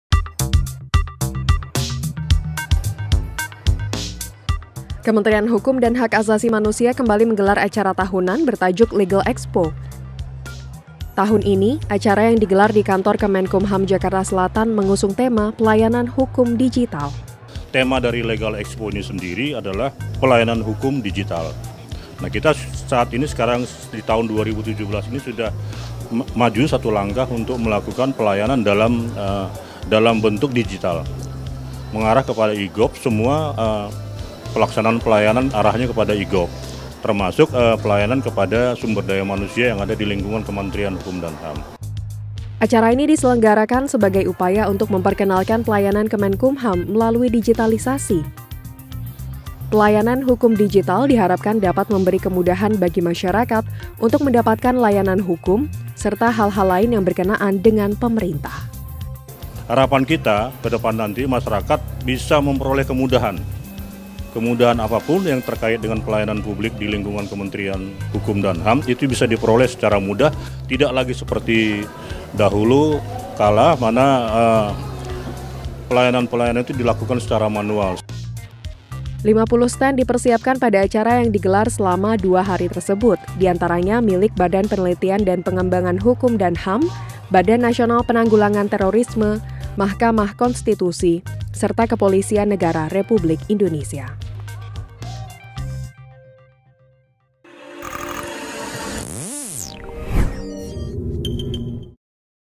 Legal Expo 2017: Pelayanan Hukum Digital
Kementerian Hukum dan Hak Asasi Manusia (Kemenkumham) menggelar acara Legal Expo 2017 yang mengusung tema Pelayanan Hukum Digital. Acara ini diselenggarakan untuk memperkenalkan pelayanan Kemenkumham melalui digitalisasi, yang diharapkan dapat memberi kemudahan bagi masyarakat untuk mendapatkan layanan hukum, serta hal-hal lain yang berkenaan dengan pemerintah.

Akses dan Follow Official Account VISUALTV.LIVE di :

- Line : @visualtvlive
- Path : @visualtvlive
- BBM Channel PIN : C002918A4